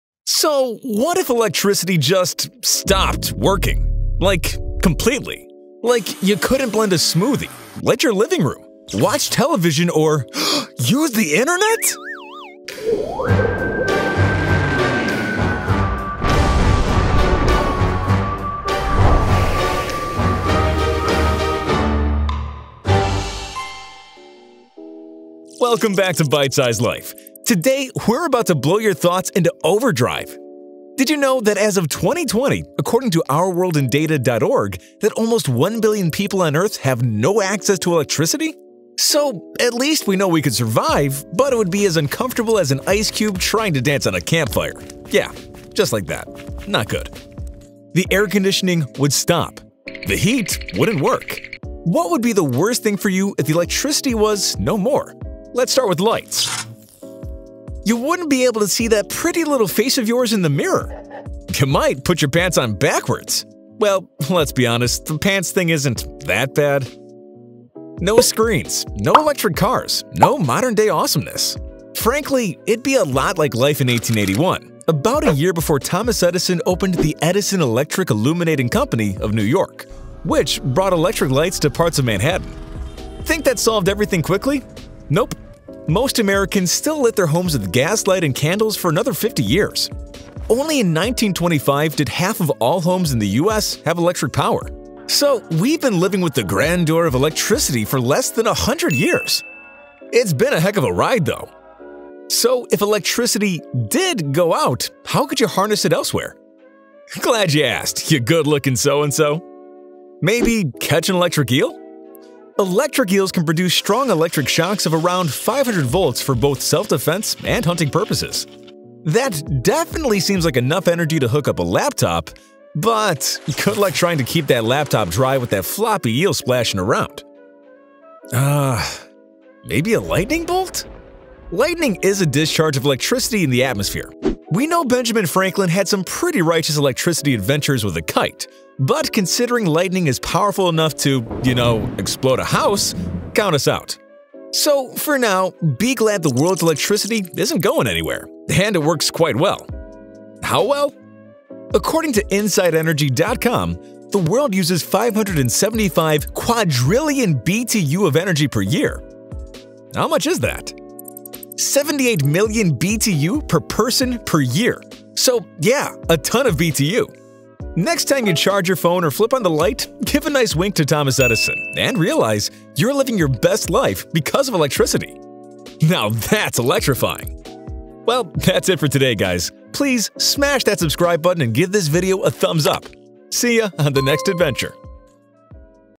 What If Earth's Electricity Just Stopped Working? | Apocalyptic Blackout
Hi! Welcome back to Bite Size Life. Today we’re about to blow your thoughts into overdrive: did you know that as of 2020, according to OurWorldInData.org, that almost one billion people on earth have no access to electricity?

So What If Earth's Electricity Just Stopped Working?

what is the entire planet’s electricity just...three...two...one….poof.